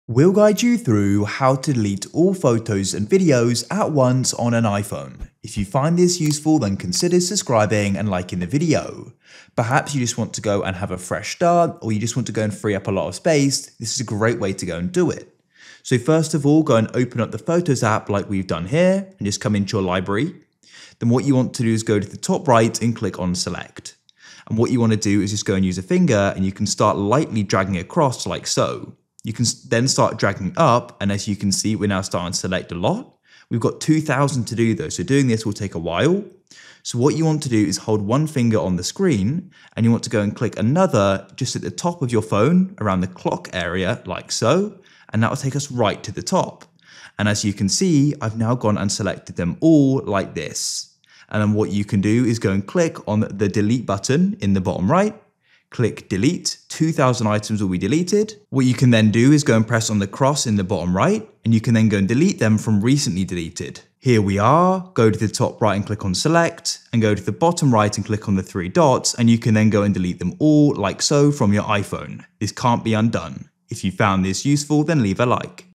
How To Delete All Photos On iPhone At Once - Full Guide
Learn how to delete all photos on iphone at once in this video.

GuideRealm